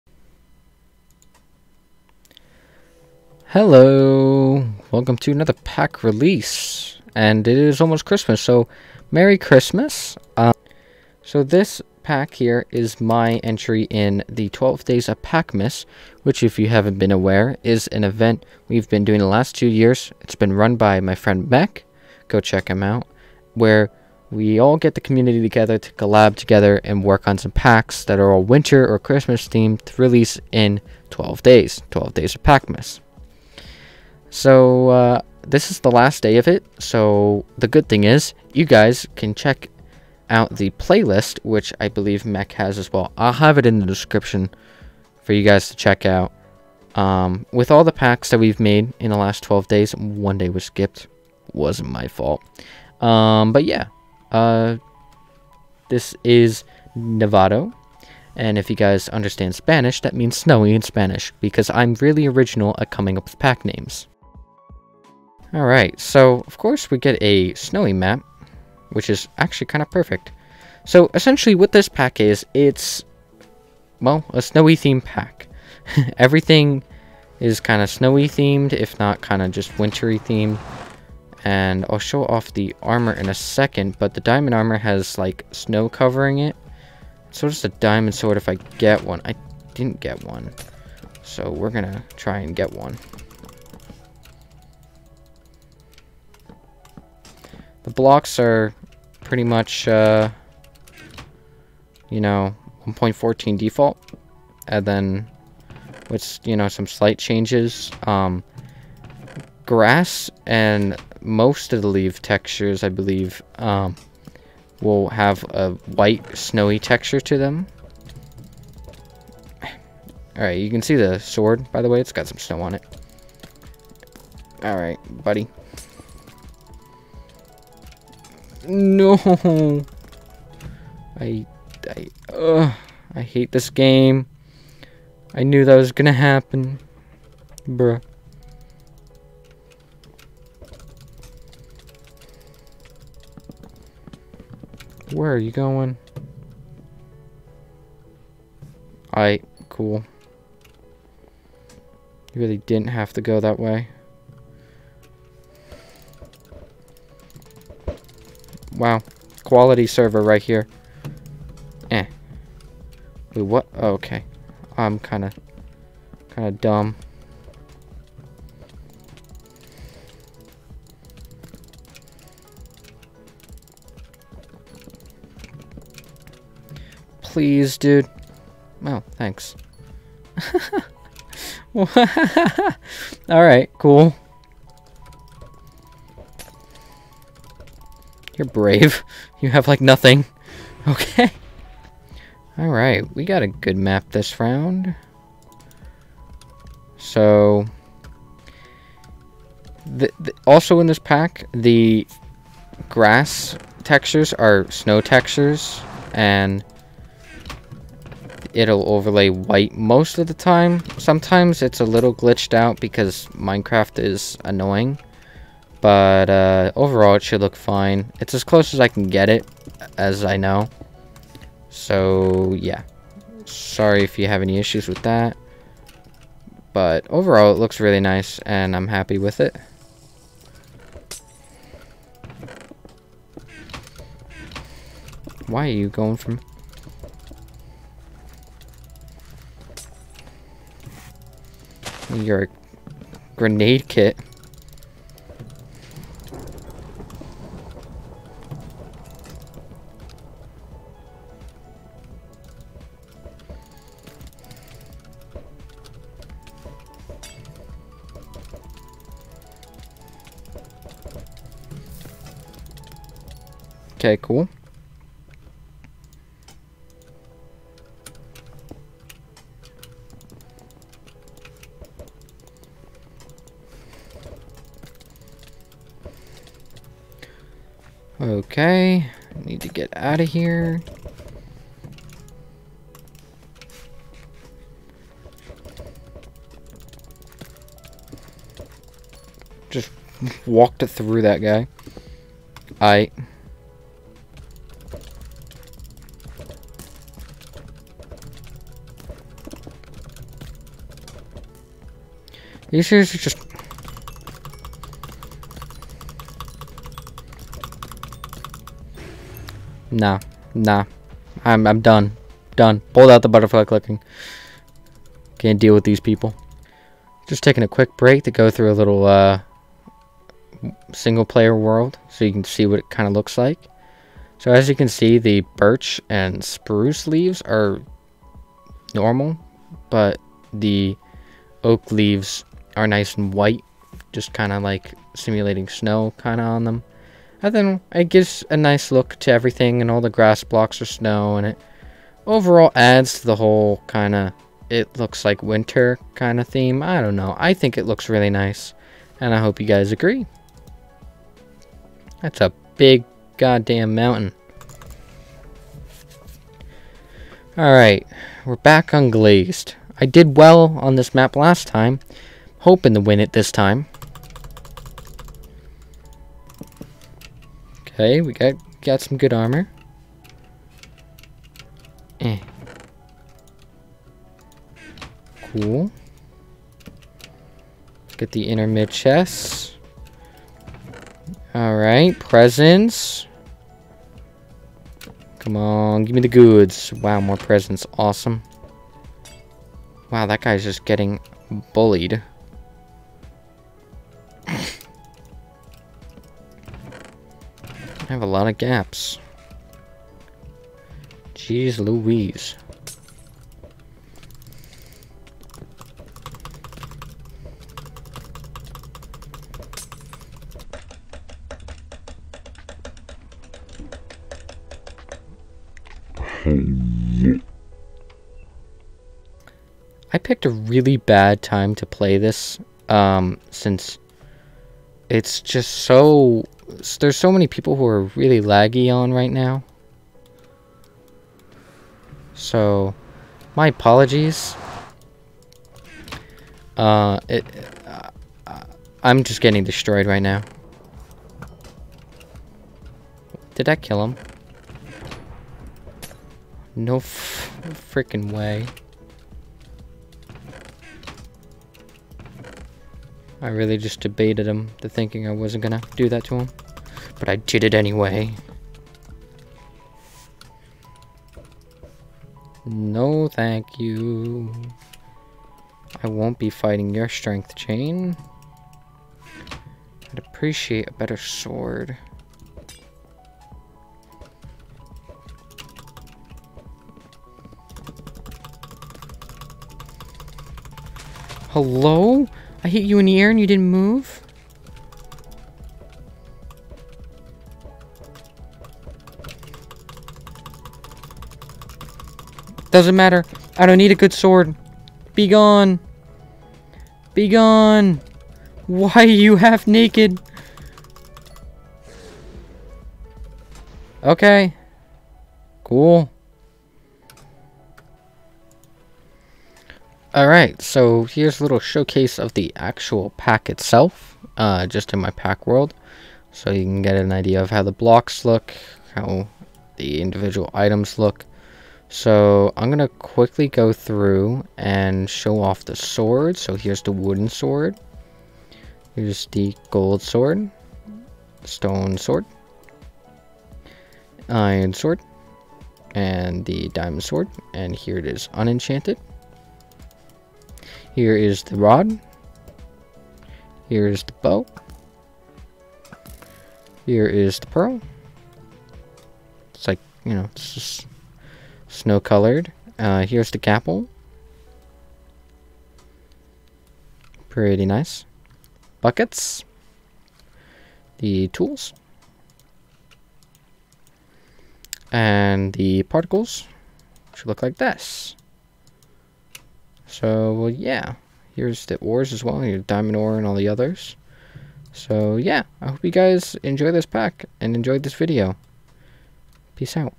nevado (16x) Winter Pack Release (12 Days of Packmas Day 12)
A winter-y pack that will make your pvp experience a lot more festive!

The Other (Awesome) Creators in 12 Days of Packmas -

FAQ -
Can you make me a pack?
No.

Whenever I can, usually a weekend at 12pm EST/EDT

Any other questions can be found in my “About” page.